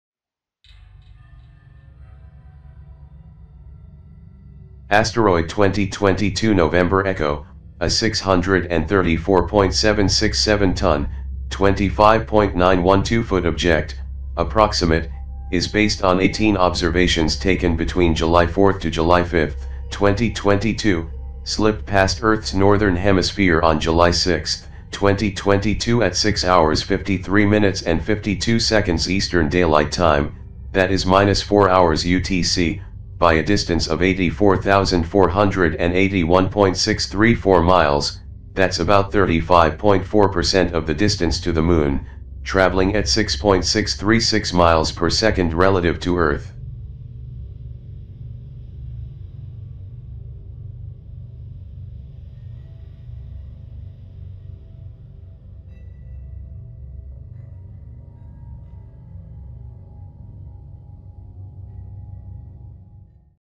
ASTEROID 2022 NE
Asteroid 2022 NE, a 634.767 ton, 25.912 foot object (approximate), is based on 18 observations taken between July 4th to July 5th 2022, slipped past earth's northern hemisphere on July 6th 2022 @ 06:53:52 Eastern Daylight Time (that is minus 4 hours UTC) by a distance of 84,481.634 miles (that's about 35.4% of the distance to the moon) travelling at 6.636 miles per second relative to earth.